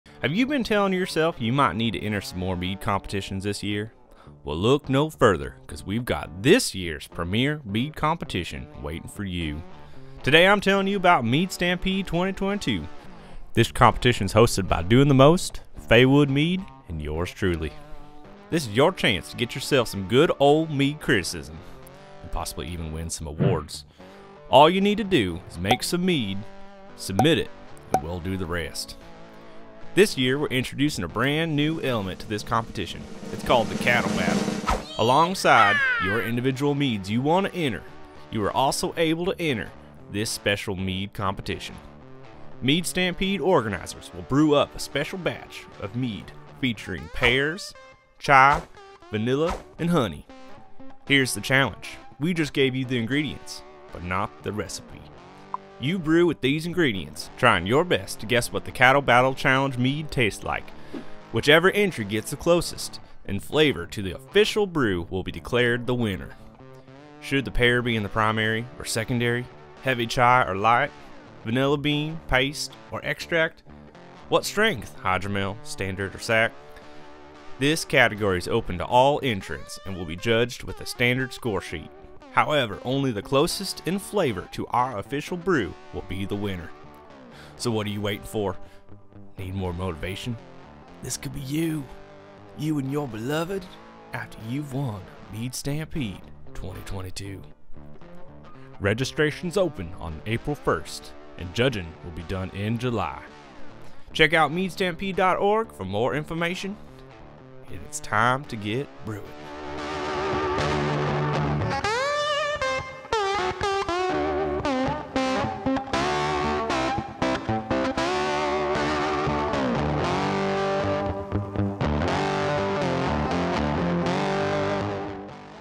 The Mead Competition You Must Enter! (Mead Stampede 2022)
Join us for Mead Stampede 2022! Entries will start on April 1st and Judging will be in July.

Mead Stampede organizers will brew up a special batch of mead featuring: pears, chai, vanilla, and honey. Here’s the challenge: we just gave you the ingredients, but not the recipe. You brew with these ingredients – trying your best to guess what the HIGH STEAKS Challenge mead tastes like. Whichever entry gets the closest in flavor to the official brew will be declared the winner!

Should the pear be in primary or secondary? Heavy chai or light? Vanilla beans, paste, or extract? What strength – hydromel, standard, or sack?

This category is open to all entrants and will be judged with a standard score sheet. However, only the mead CLOSEST in flavor to our official brew will be the winner!